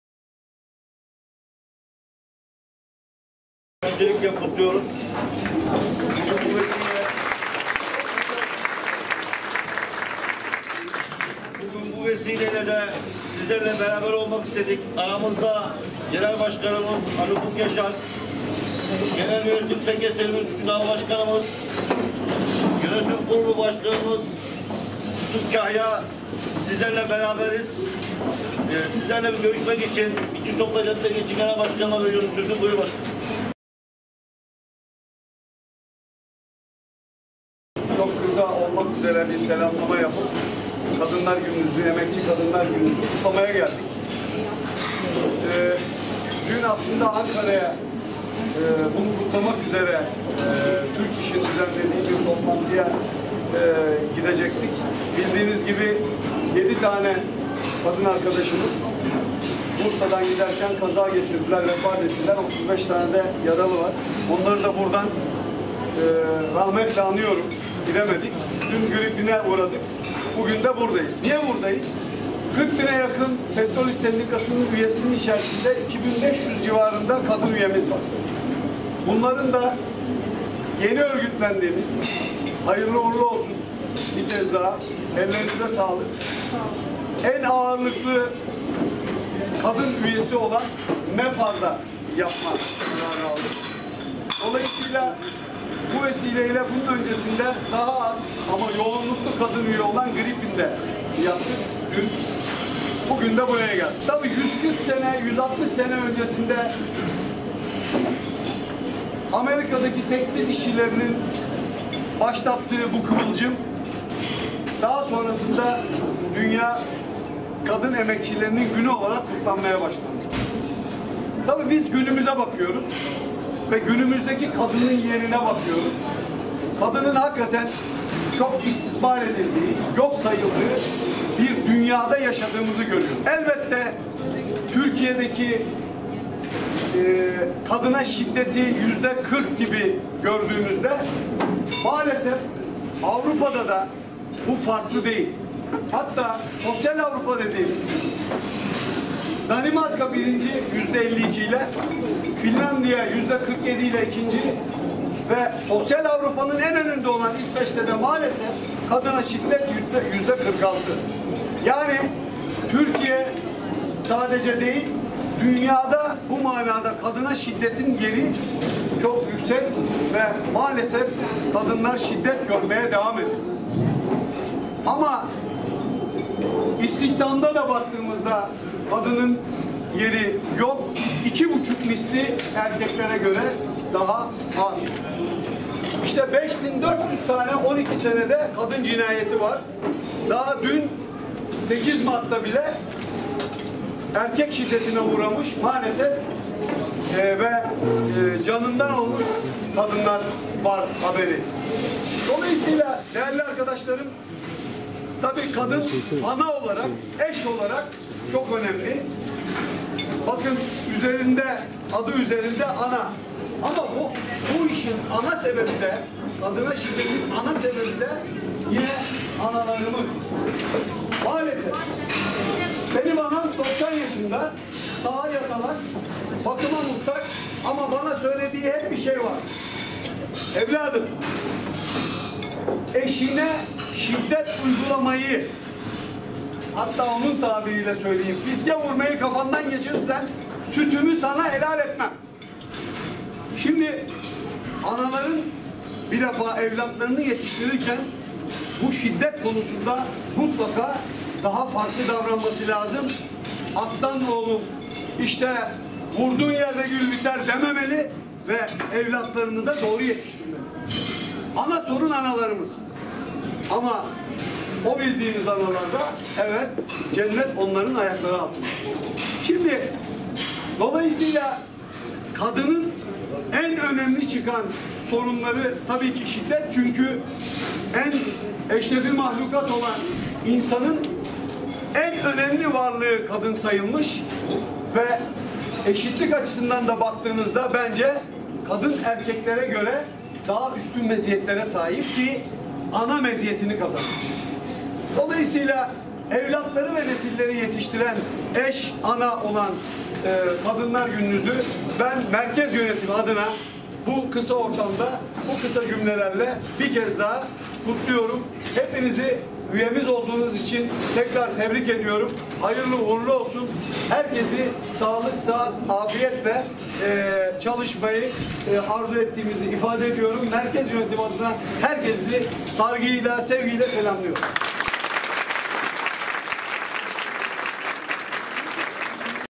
Merkez Yöneticilerimizden Mefar İlaç İşyeri’nde 8 Mart Kutlaması
Genel Başkanımız Ali Ufuk Yaşar ve Genel Yönetim Sekreterimiz Ünal Akbulut, Denetim Kurulu Başkanımız Yusuf Ziya Kahyaoğlu 8 Mart Dünya Kadınlar Günü nedeniyle yeni örgütlendiğimiz ve çok sayıda kadın üyemizin çalıştığı İstanbul 2 Nolu Şubemiz kapsamında olan Mefar İlaç İşyerini ziyaret etti.